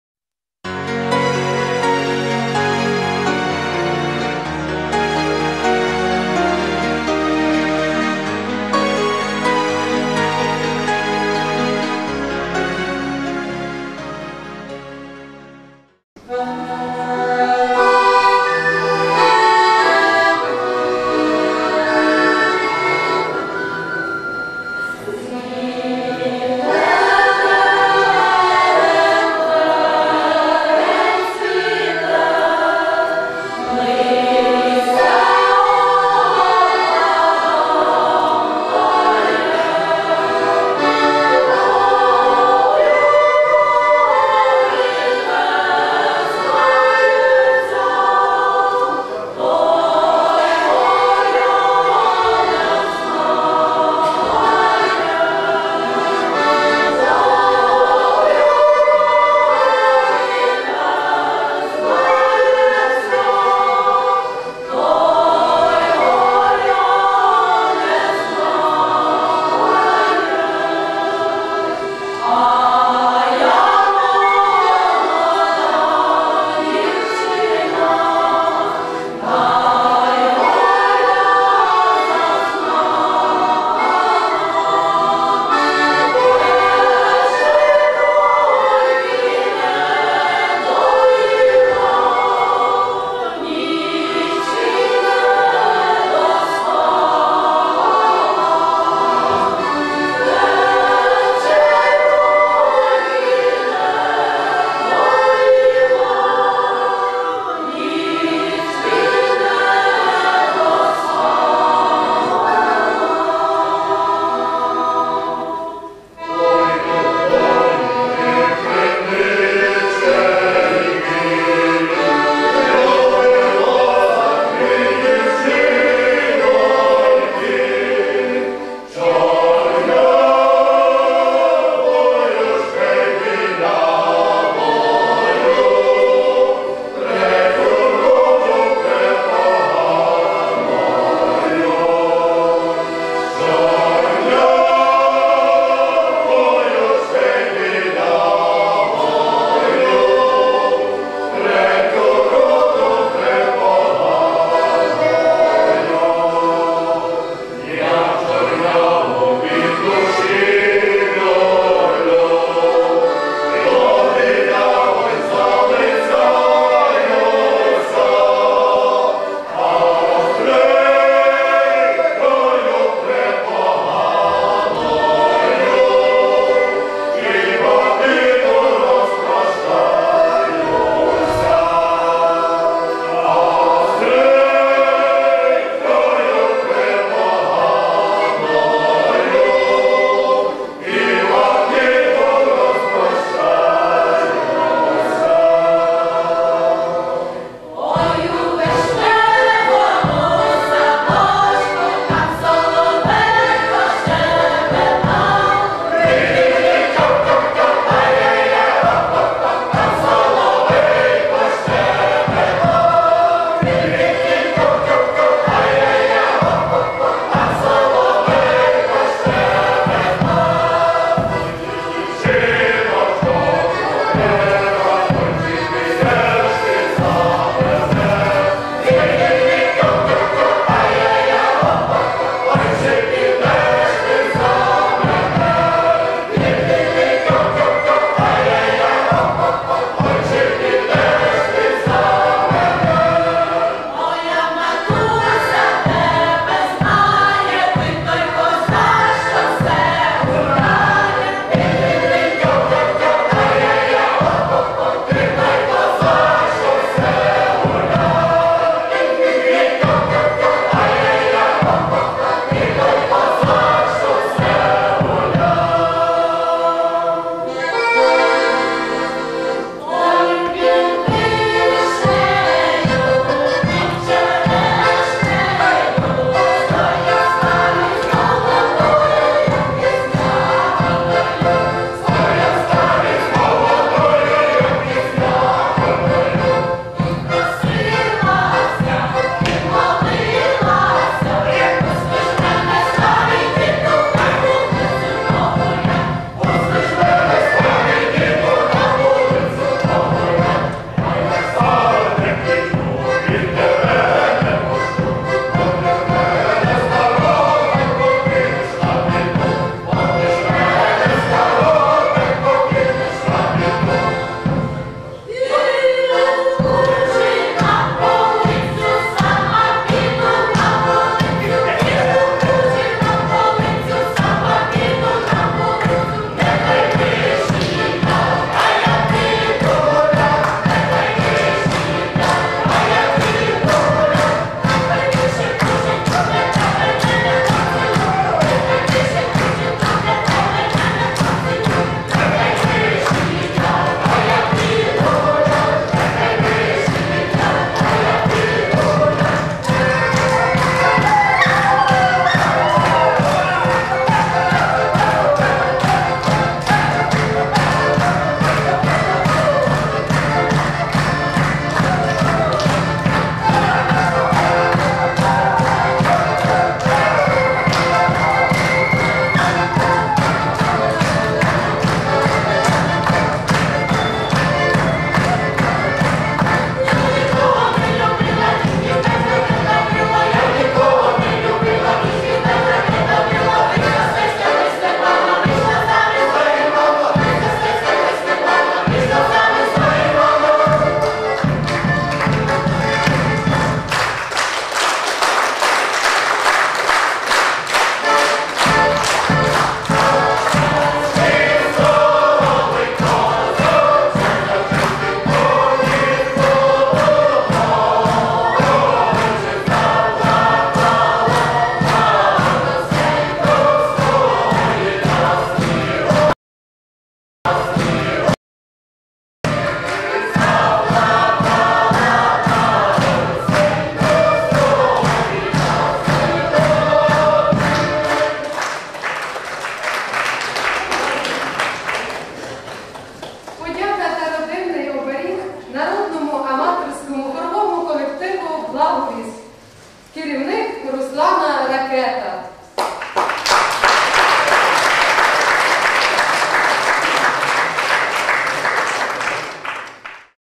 Фестиваль у Тисмениці.ч-19. с.Слобода.Франківщина.
26 вересня у РБК міста Тисмениця відбувся фестиваль народних аматорських колективів під назвою "Душі людської відкриття", в якому взяло участь більше 20 творчих груп з усіх куточків однойменного району.
    Народний аматорський жіночий вокальний ансамбль "Джерело" села
Підлісся, аматорські фольклорні колективи сіл Марківці, Одаї, Милування,
Вільшаниця, Колодіївка, Нові Кривотули, Клубівці, Липівка, Хом"яківка,
Барезівка, вокальний дует сестер Хмілів з Черніїва, вокальний ансамбль
дівчат БК с.Драгомирчани, чоловічий вокальний ансамбль БК с.Старий
Лисець, дует у складі Марії Костишин та Наталії Мицканюк, БК с. Загвіздя,
жіночі вокальні ансамблі БК селища Лисець та с. Красилівка,
аматорський колектив с. Угринів Горішній, фольклорний колектив
"Горицвіт" БК с.Узин, "Благовіст" с.Слобода,  хоровий колектив с.Підлужжя, - такому розмаїттю учасників та номерів могла б позаздрити
і столична сцена, не кажучи вже про обласну.
   Та , на жаль, це прекрасне свято побачили (якщо не враховувати оператора студії М.Бойчука та ще десяток мешканців Тисмениці), тільки його учасники та організатори.Жаль, що місцеве телебачення спромоглося зняти тільки невеличкий сюжет на місцеві новини і не зафіксувало цієї події цілком, що не робить честі ні його керівнику п.Володимиру Струку, ні місцевій владі.
    Замовлення відео з фестивалю:Ів-Франківськ,вул.Степана Бандери-6.